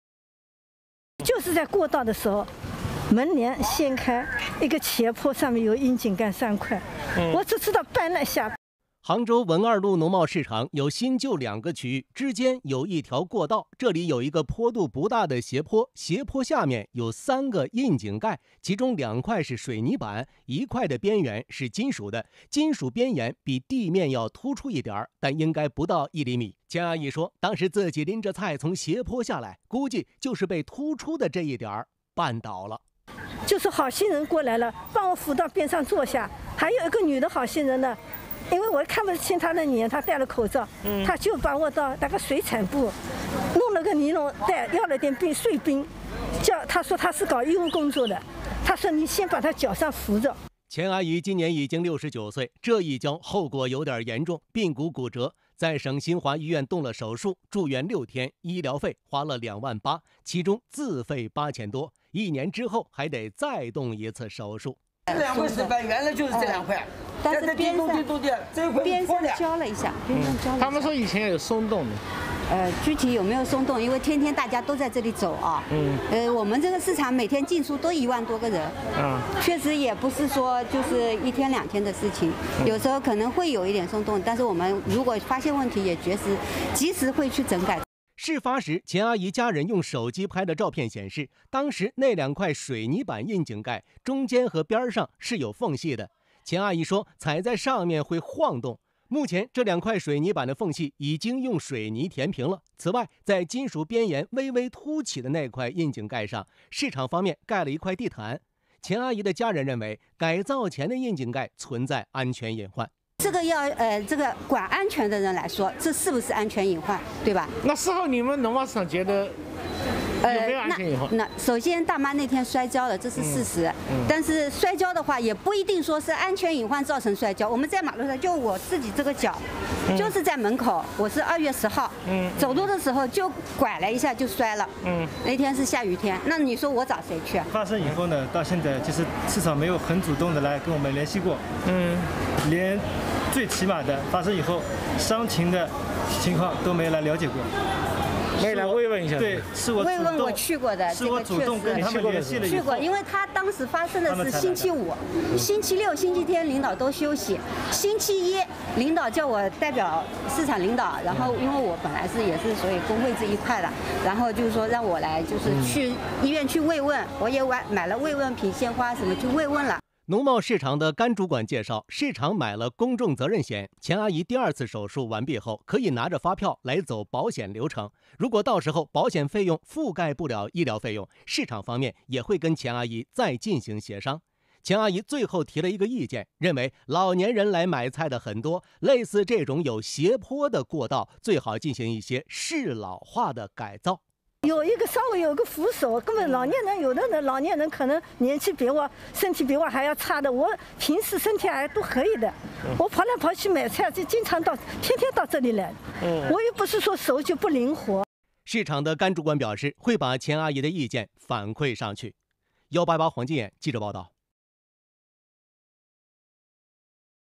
【1818黄金眼】阿姨感谢好心人，也给菜场提意见
【阿姨感谢好心人 也给菜场提意见】69岁的钱阿姨在杭州文二路农贸市场摔了一跤，髌骨骨折，动了手术，一年之后还得再动手术。她说想感谢当时伸出援手的好心人，也想给市场提提意见。

新闻标签：